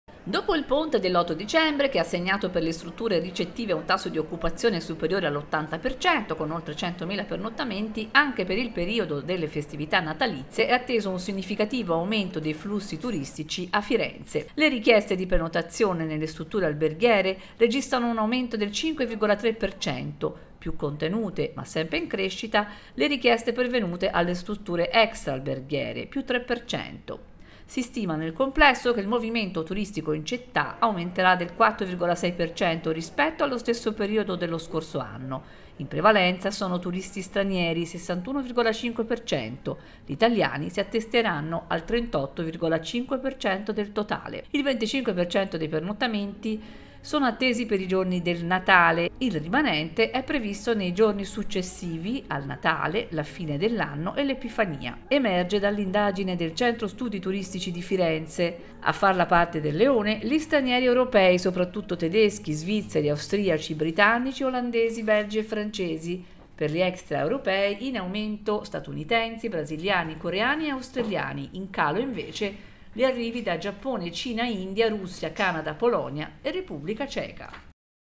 2023-12-21 FIRENZE - 500MILA PRESENZE TURISTICHE ATTESE PER LE FESTE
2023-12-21 FIRENZE - 500MILA PRESENZE TURISTICHE ATTESE PER LE FESTE